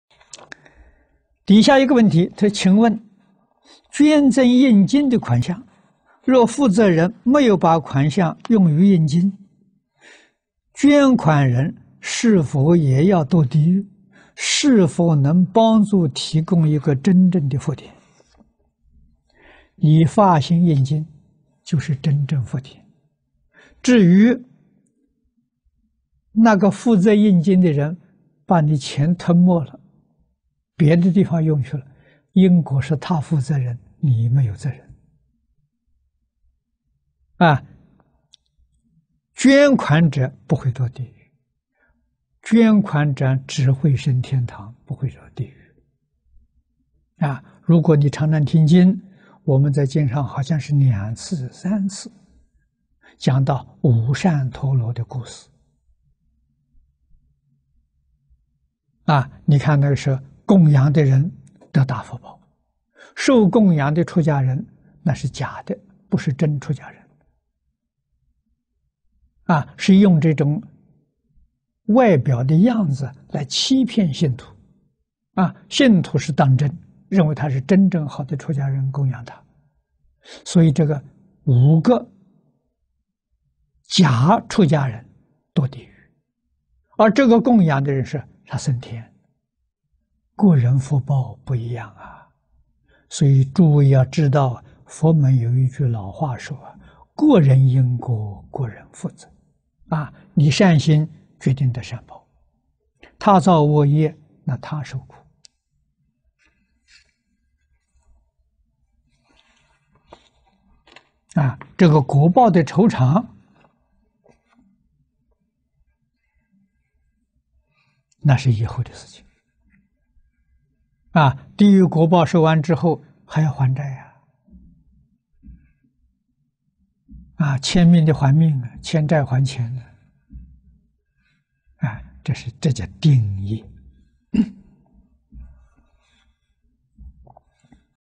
淨空老法師:請問捐贈印經的款項後,若負責人沒有把款項用於印經,捐款人是否也要墮地獄？是否能幫助提供一個真正的福田？
節錄自—學佛答問（答香港參學同修之八十九）

影片字幕：
問：底下一個問題，請問捐贈印經的款項後，若負責人沒有把款項用於印經，捐款人是否也要墮地獄？是否能幫助提供一個真正的福田？
答：你發心印經就是真正福田，至於負責印經的人把你的錢吞沒了，別的地方用去了，因果是他負責任，你沒有負責任。捐款者不會墮地獄，捐款者只會生天堂，不會下地獄。如果你常常聽經，我們在經上好像是兩次、三次，講到五扇提羅的故事，你看那個時候供養的人得大福報。受供養的出家人那是假的，不是真出家人，是用這種外表的樣子來欺騙信徒。信徒是當真，認為他是真正好的出家人，供養他，所以這五個假出家人墮地獄；而這個供養的人，她生天，各人福報不一樣。所以諸位要知道佛門有句老話說，「各人因果，各人負責」，你善心決定得善報，他造惡業那他受苦。果報的酬償，那是以後的事情，地獄果報受完之後，還要還債，欠命的還命，欠債還錢，這叫定業。